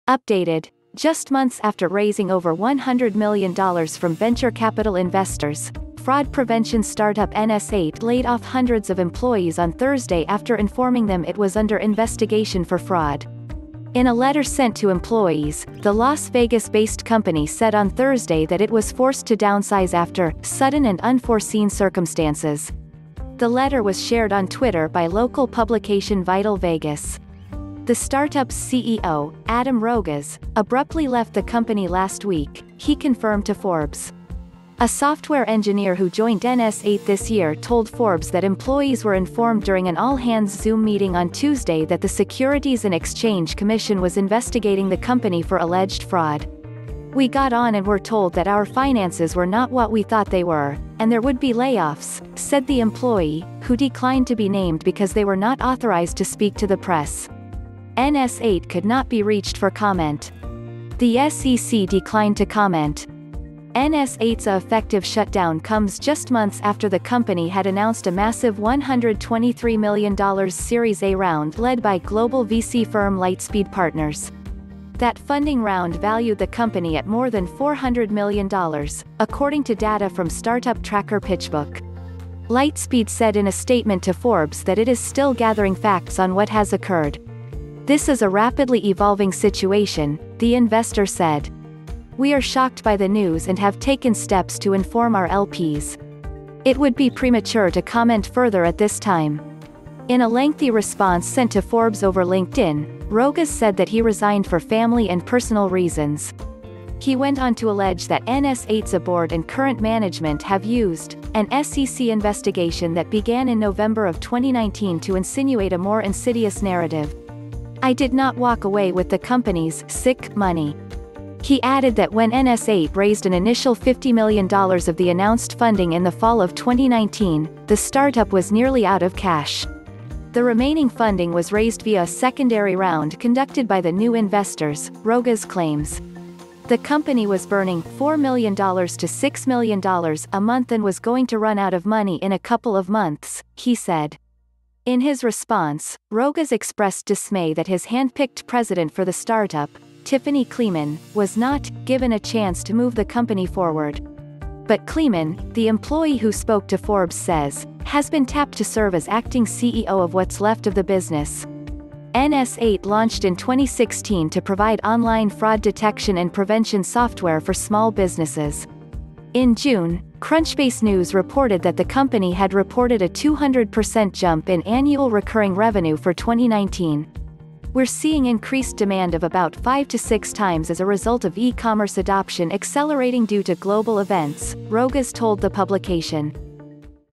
Fraud Prevention Startup NS8 Lays Off Hundreds CEO Departs Amid SEC
Updated: Just months after raising over $100 million from venture capital investors, fraud prevention startup NS8 laid off hundreds of employees on Thursday after informing them it was under investigation for fraud. In a letter sent to employees, the Las Vegas-based company said on Thursday that it was forced to downsize after “sudden and unforeseen circumstances.” The letter was shared on Twitter by local publication Vital Vegas. The startup’s CEO, Adam Rogas, abruptly left the company last week, he confirmed to Forbes. A software engineer who joined NS8 this year told Forbes that employees were informed during an all-hands Zoom meeting on Tuesday that the Securities and Exchange Commission was investigating the company for alleged fraud. “We got on and were told that our finances were not what we thought they were, and there would be layoffs,” said the employee, who declined to be named because they were not authorized to speak to the press. NS8 could not be reached for comment. The SEC declined to comment. NS8’s effective shut down comes just months after the company had announced a massive $123 million Series A round led by global VC firm Lightspeed Partners. That funding round valued the company at more than $400 million, according to data from startup tracker PitchBook. Lightspeed said in a statement to Forbes that it is still gathering facts on what has occurred. “This is a rapidly evolving situation,” the investor said. “We are shocked by the news and have taken steps to inform our LPs. It would be premature to comment further at this time."In a lengthy response sent to Forbes over LinkedIn, Rogas said that he resigned for family and personal reasons. He went on to allege that NS8’s board and current management have used “an SEC investigation that began in November of 2019 to insinuate a more insidious narrative... I did not walk away with the companies [sic] money.”He added that when NS8 raised an initial $50 million of the announced funding in the fall of 2019, the startup was nearly out of cash. The remaining funding was raised via a secondary round conducted by the new investors, Rogas claims. “The company was burning [$4 million to $6 million] a month and was going to run out of money in a couple of months,” he said. In his response, Rogas expressed dismay that his handpicked president for the startup, Tiffany Kleemann, was not “given a chance to move the company forward.” But Kleemann, the employee who spoke to Forbes says, has been tapped to serve as acting CEO of what’s left of the business. NS8 launched in 2016 to provide online fraud detection and prevention software for small businesses. In June, Crunchbase News reported that the company had reported a 200% jump in annual recurring revenue for 2019. “We’re seeing increased demand of about five to six times as a result of e-commerce adoption accelerating due to global events,” Rogas told the publication.